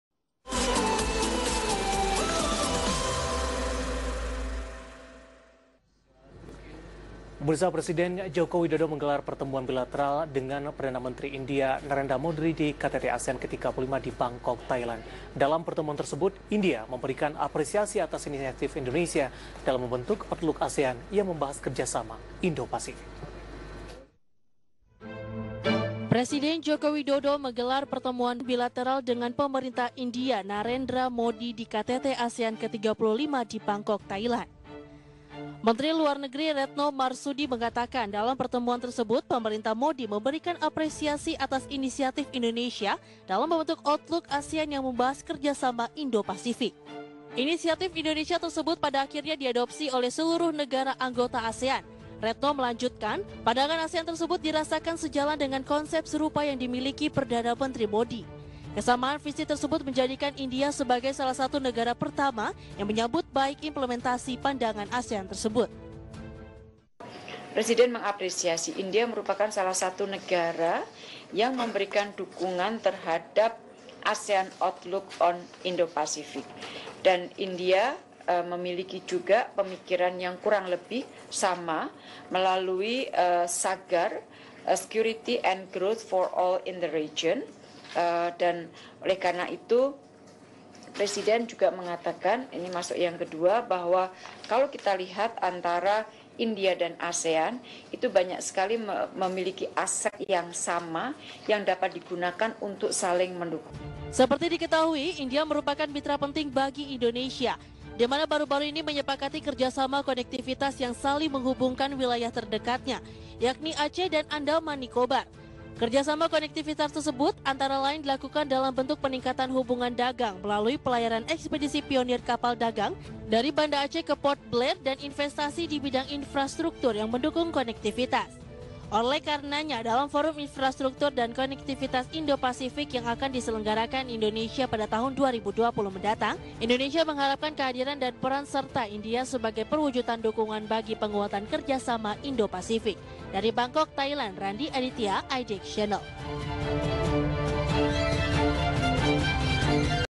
DATANGI KTT ASEAN, INI ALASAN INDONESIA JALIN HUBUNGAN MESRA DENGAN INDIA
IDX CHANNEL - Presiden Joko Widodo (Jokowi) mengapresiasi dukungan India sebagai salah satu negara pertama yang menyatakan kesiapan untuk mengimplementasikan kerja sama Indo-Pasifik. Jokowi pun mengajak India untuk mempercepat implementasi ASEAN Outlook on the Indo-Pacific (AOIP).